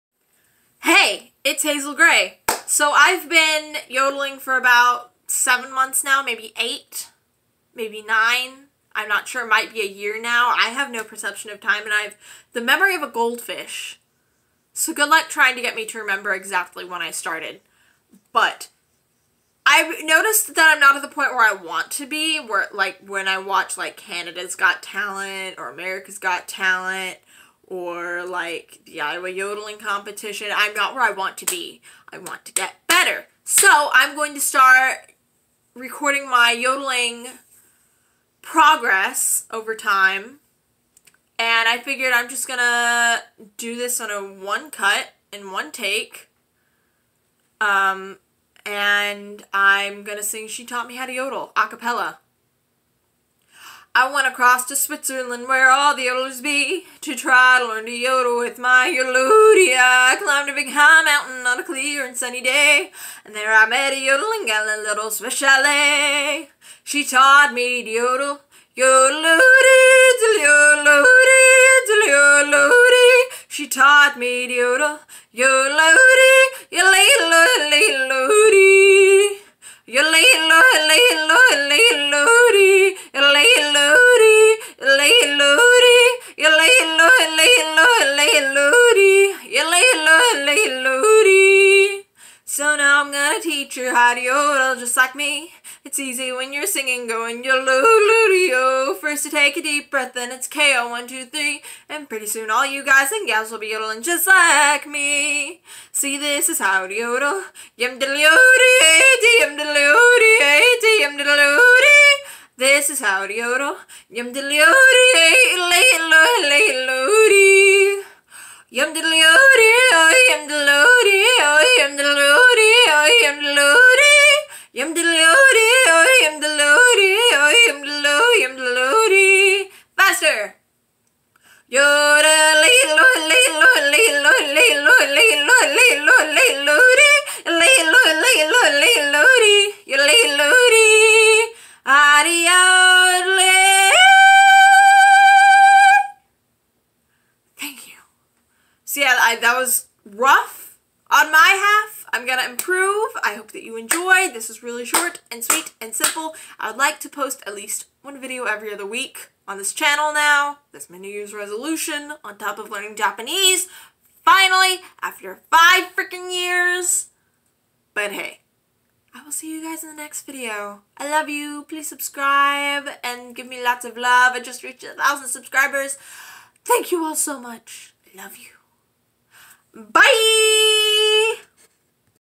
Yodeling Progress #1
I'm going to be following my yodeling progress over time.  This is the first of many videos.